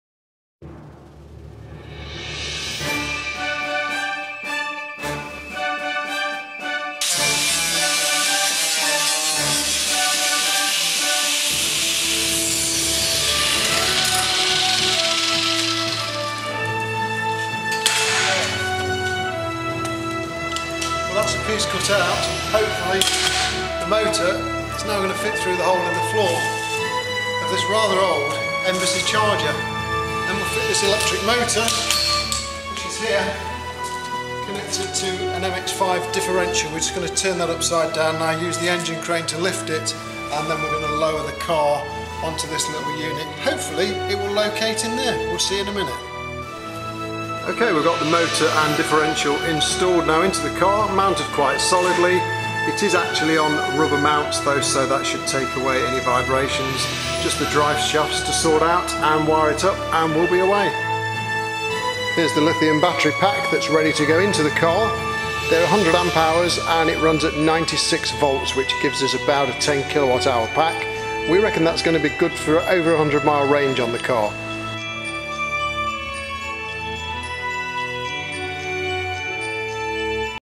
VW BEETLE GOES ELECTRIC
You see here an old EBC Charger that is VW Beetle based being converted to electric power using Lithium Yttrium batteries and an AC brushless motor with regenerative braking and a programmed controller. Batteries are maintained by a bespoke BMS.